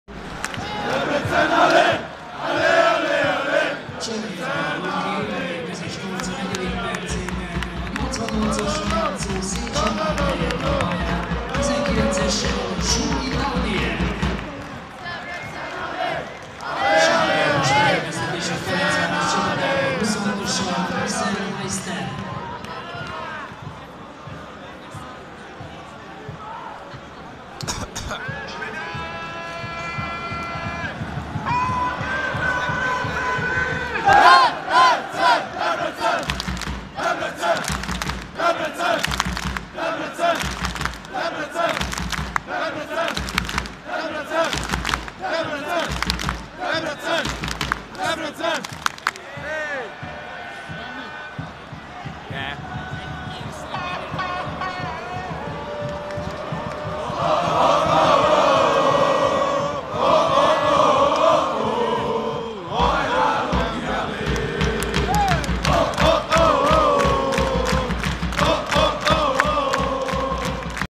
DVSC-FTC 2-1 A Loki-ultrák szurkolnak 8.rész
A Loki-ultrák buzdítják csapatukat. Ahogyan én láttam...